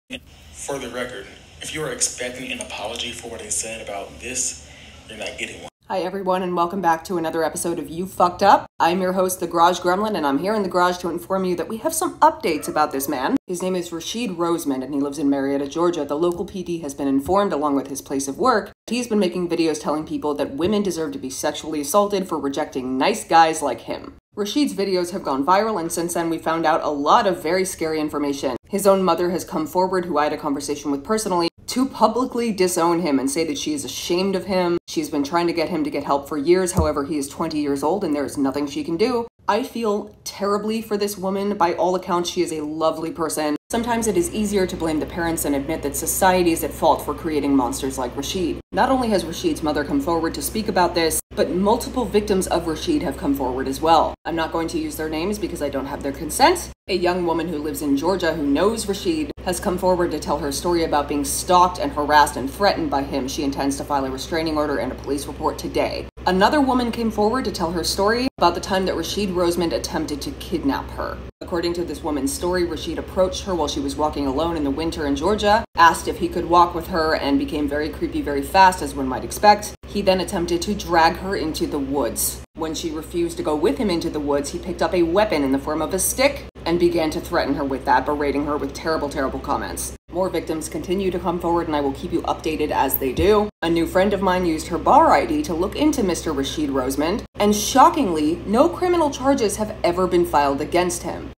Breaking News!!! Rashid Went Viral For Saying Women Deserved To Be R*ped If They Reject Men
Breaking News!!
Rashid; who resides in Georgia "Went Viral" on TikTok for saying women deserved to be R*ped if they reject "Nice Guys" Like him. This shared video is from a user on TikTok called The Garage Gremlin, who did thorough research on Rashid.
Upon finding out her son's viral video, his mother has since disowned Rashid, and says he needs help, yet tried to no avail, and because he is twenty years old, she cannot do anything else, since he is in fact an adult.
Rashid has assaulted multiple women, yet has not been charged for his crimes, and if you are female/have a uterus, and see Rashid, please know men like this get off on sexually assaulting women, and forcefully impregnating them, hence this is a safety awareness video for anyone who has a uterus.